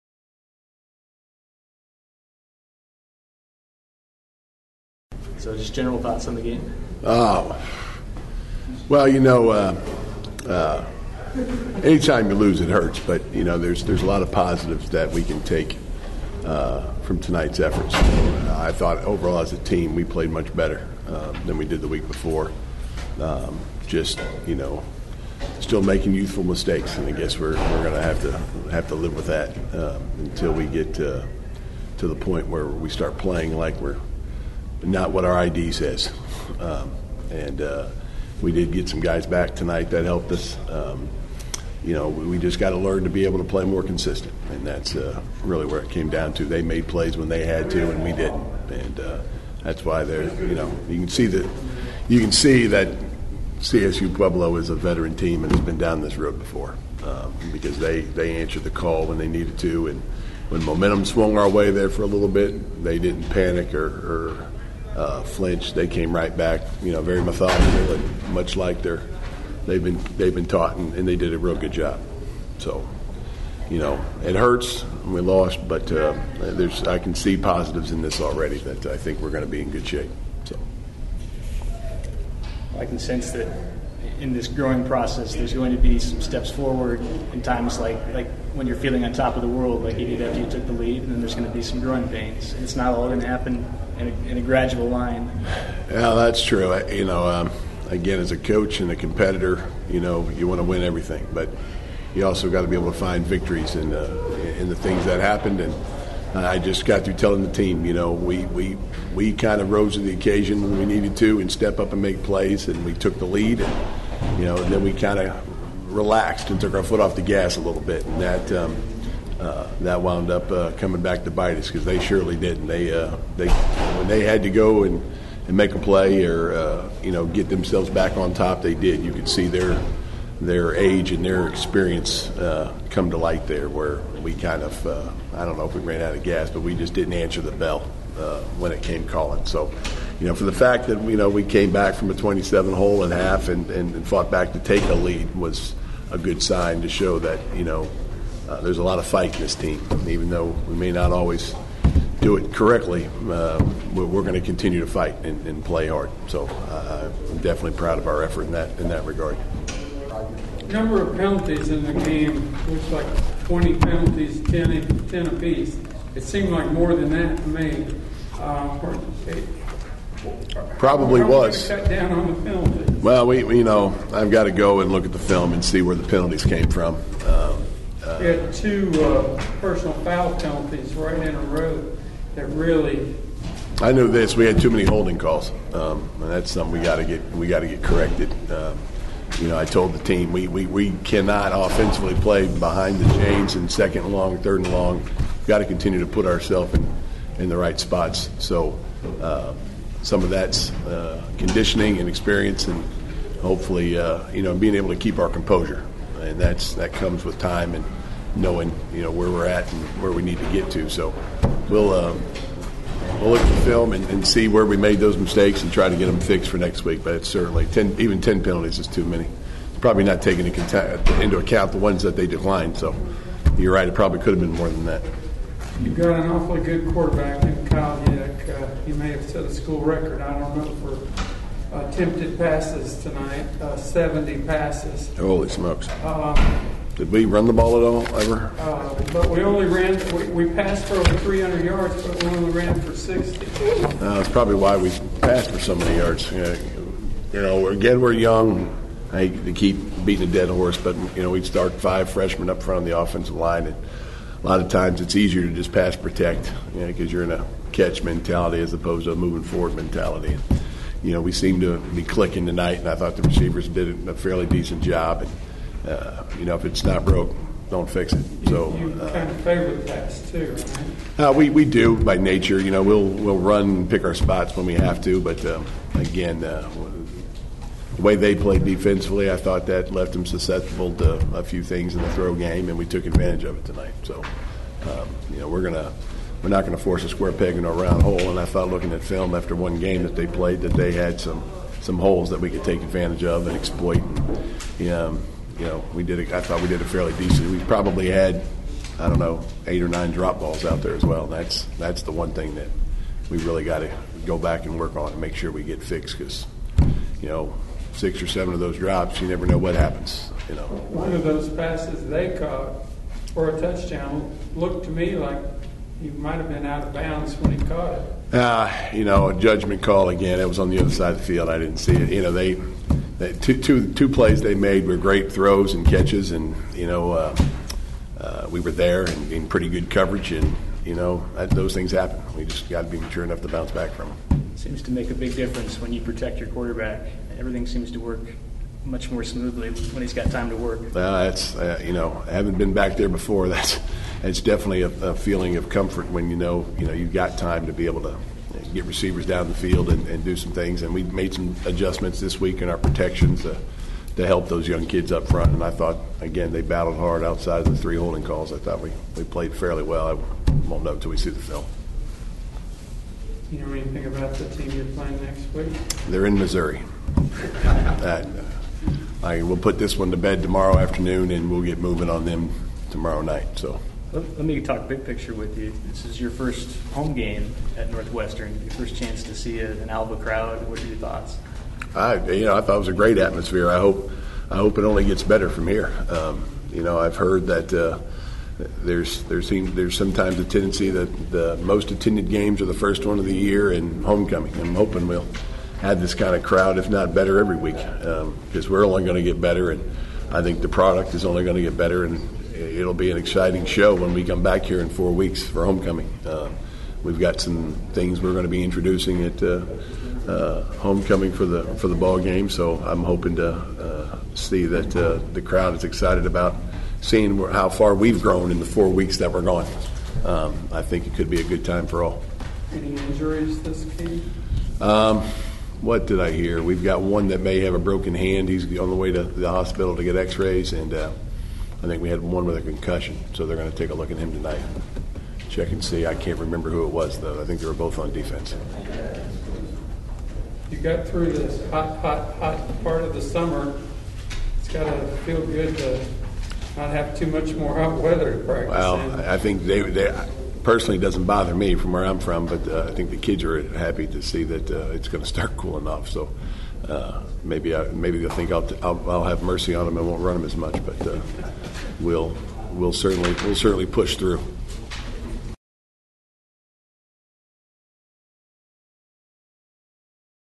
Coach Alan Hall Discusses Ranger Loss
The Rangers seemed much improved over last week's game. And for awhile, led in the contest against Colorado State, Pueblo.